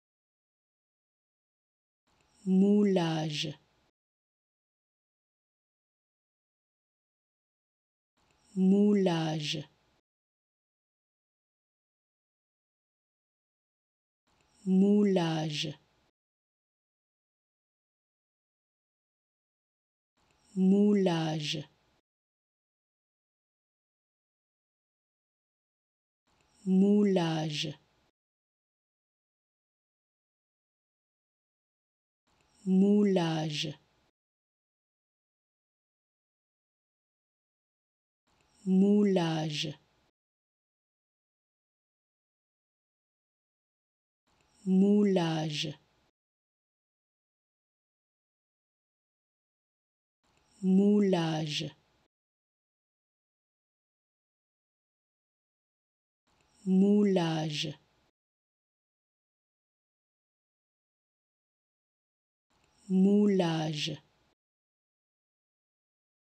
How to Pronounce ''Moulage'' (Moulding) Correctly in French
Learn how to say and properly pronounce ''Moulage'' in French with this free pronunciation tutorial. Apprenez à prononcer des mots en français grâce à nos tutoriels.